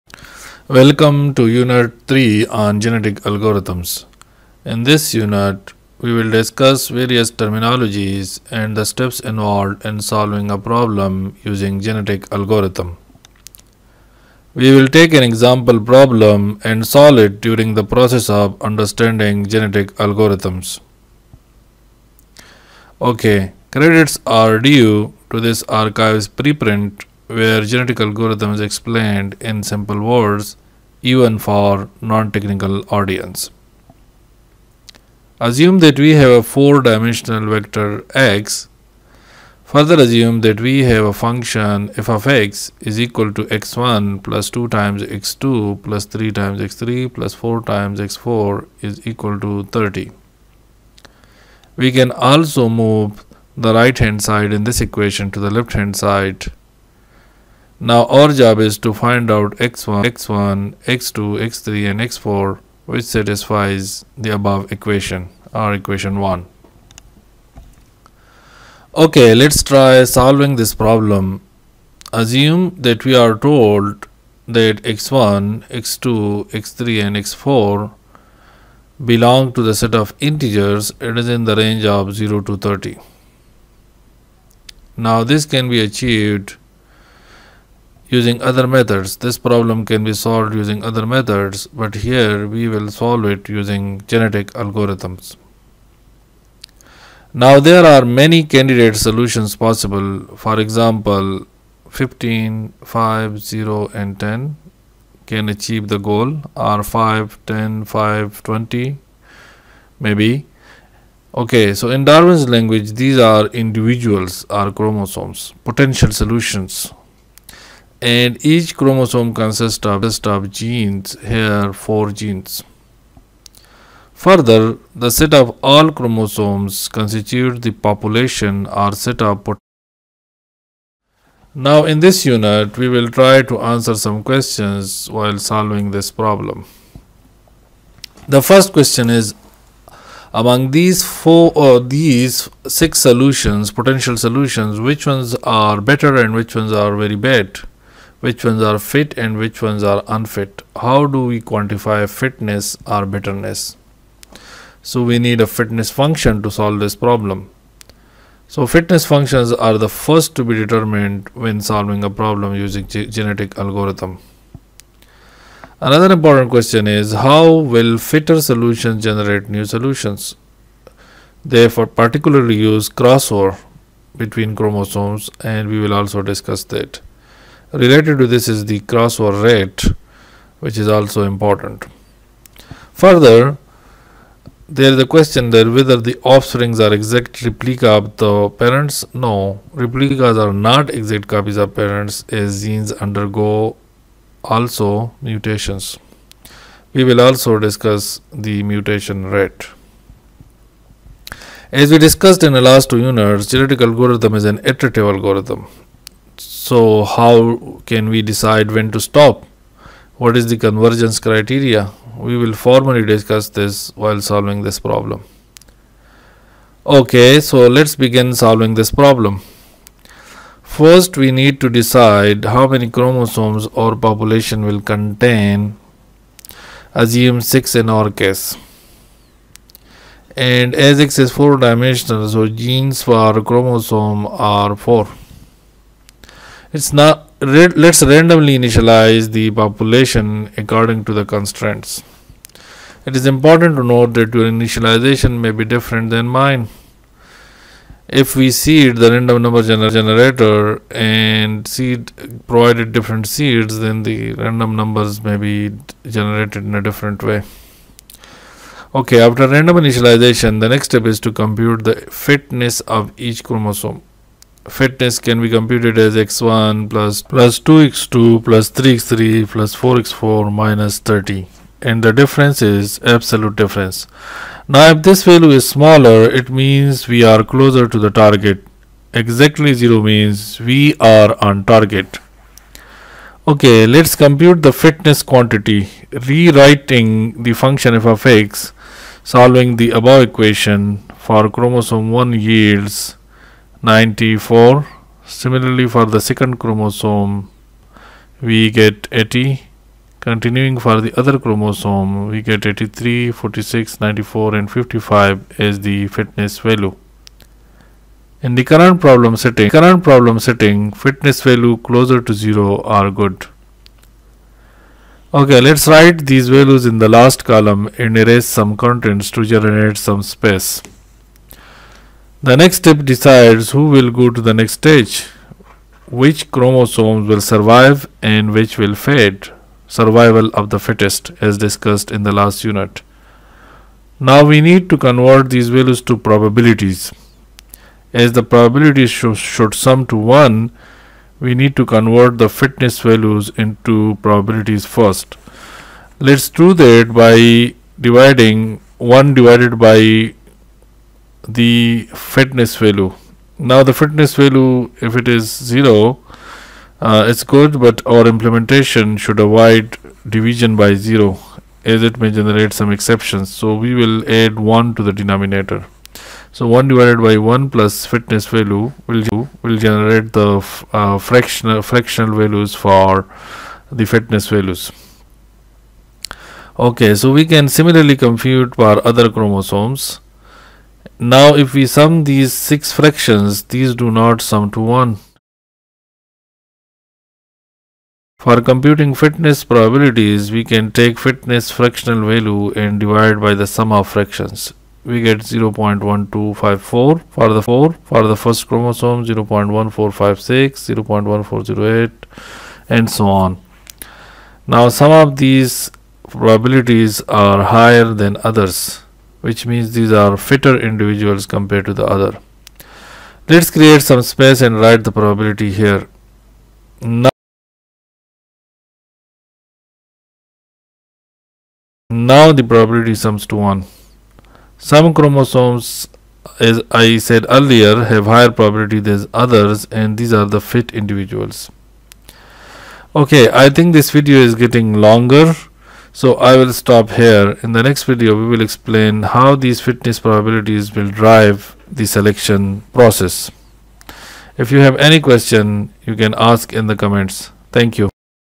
Solving Simple Math Equality Problem Using Genetic Algorithm
CS351-AI Lecture 26 Genetic Algorithm Unit3 (Fitness Function)